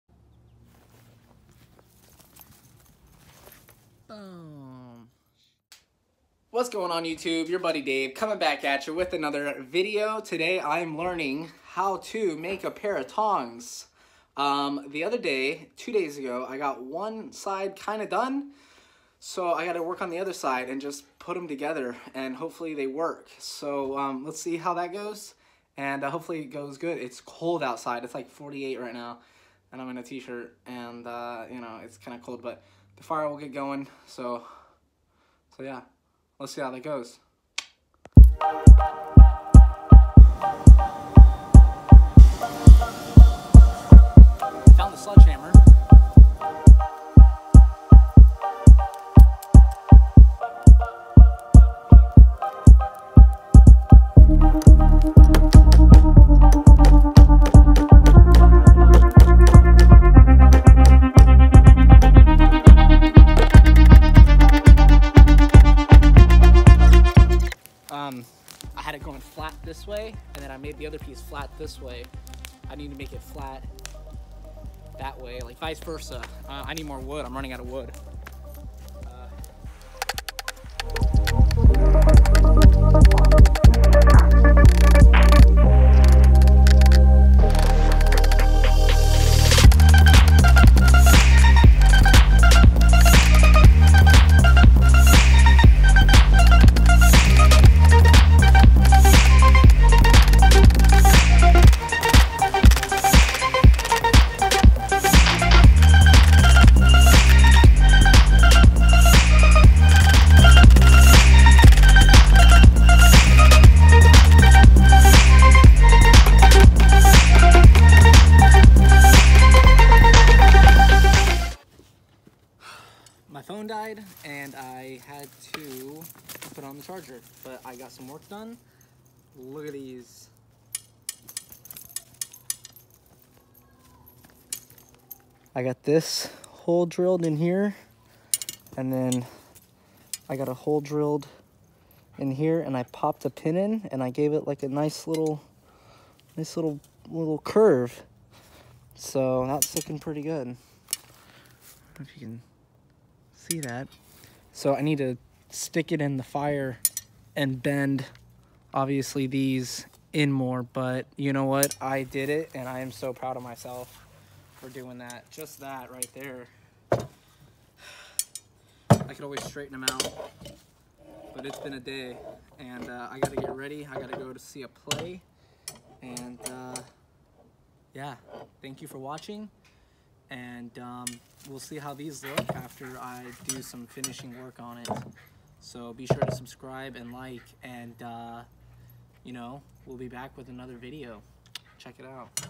My First Forged Blacksmith Tongs
In this video I took a stab at making my own blacksmith tongs out of rebar.